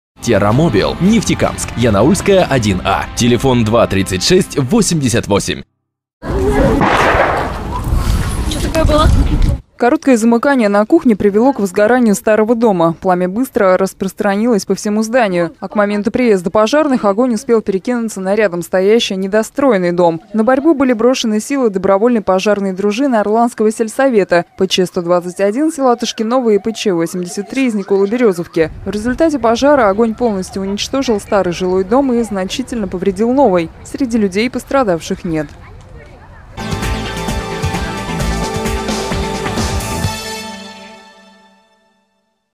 Из искры разгорится пламя
Электрочайник стал виновником беды. В пятницу в полдень в селе Арлан Краснокамского района сгорела два жилых дома. Оба здания были расположены на одном участке.